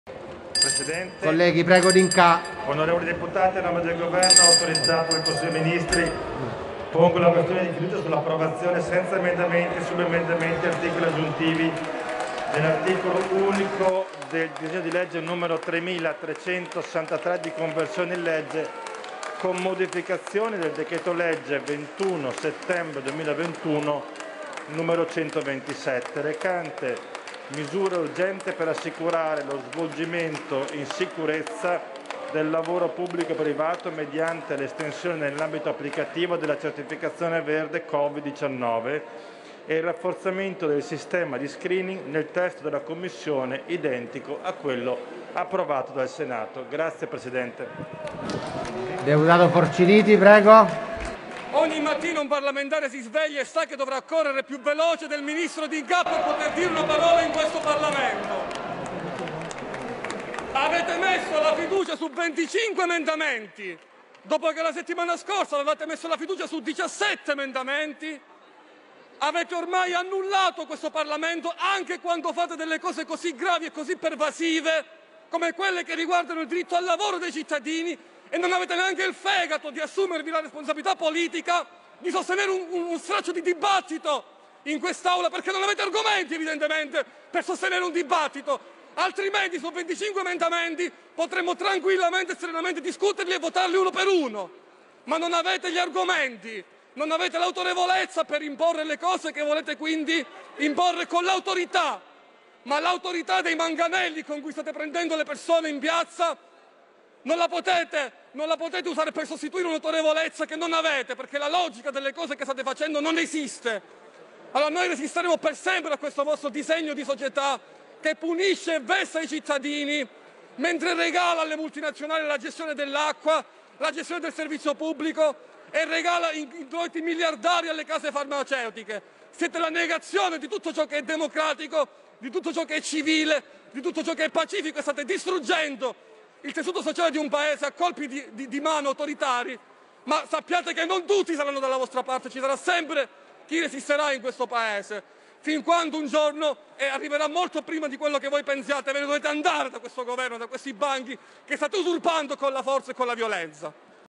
CAOS ALLA CAMERA: FORCINITI È UNA FURIA CONTRO D'INCÀ ▷ "VOI SIETE LA NEGAZIONE DELLA DEMOCRAZIA!"
Dl Green pass: posta la fiducia anche alla camera dopo che il testo è passato al Senato la scorsa settimana. Il decreto che "contiene le misure urgenti per assicurare lo svolgimento in sicurezza del lavoro pubblico e privato mediante l'estensione dell'ambito applicativo della certificazione verde Covid 19 e il rafforzamento del sistema di screening" è passato integralmente, come ha fatto sapere il Ministro per i Rapporti con il Parlamento D'Inca.
Respinte in avvio le questioni pregiudiziali di costituzionalità alla discussione del decreto legge depositate dalla componente del gruppo misto-L'alternativa c'è e da Fdi.

Non l'hanno presa bene i rispettivi gruppi parlamentari, che hanno parlato subito dopo D'Inca mostrando il loro disappunto, primo tra i quali Francesco Forciniti de "L'Alternativa c'è" interpellato a caldo dal presidente Fico.
Ecco il suo intervento.